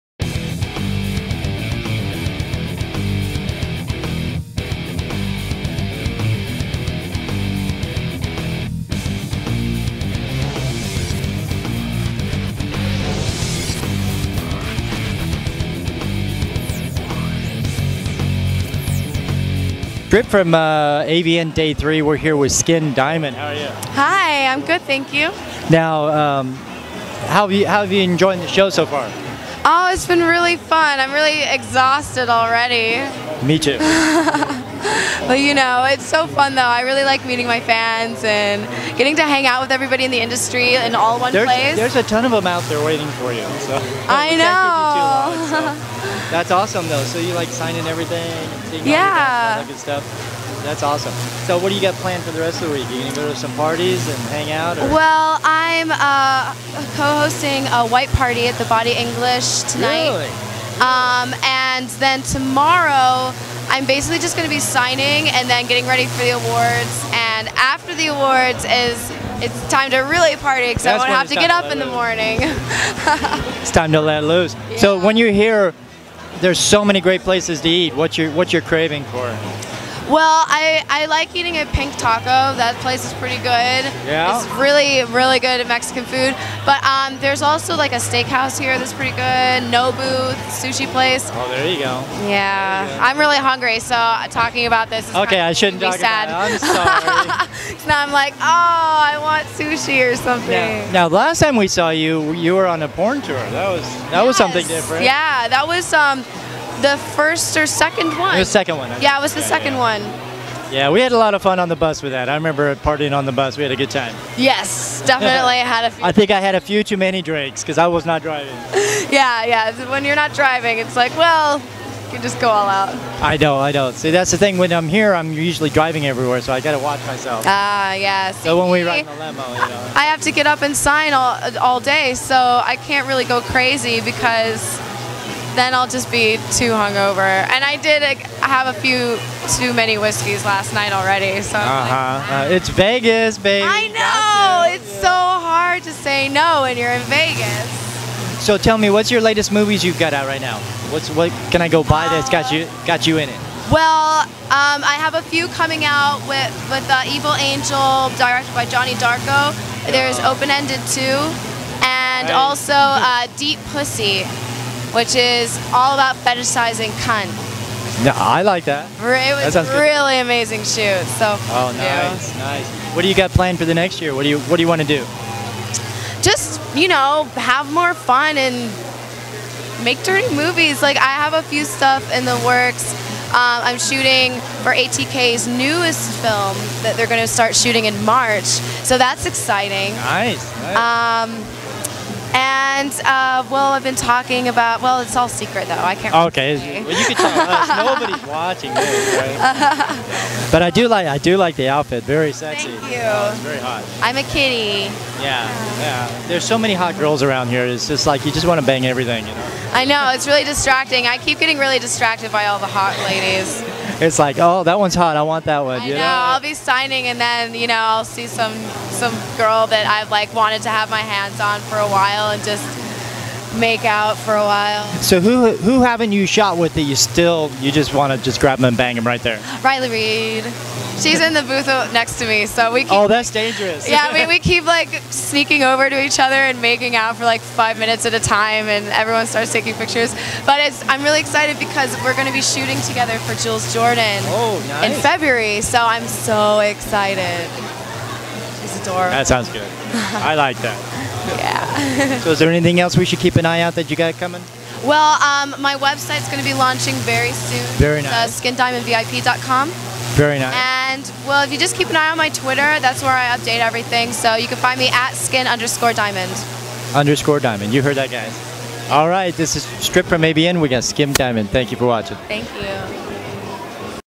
Skin Diamond AVN 2013
This time around we had the pleasure of speaking with Skin Diamond.   We met Skin back in LA for the porn tours, and got to catch up with her this year at AVN in the Evil Angel both.  Now Skin is a blast, and really down to earth as you will see, besides being a hottie for sure.

| Striptaculous - Inteviews and show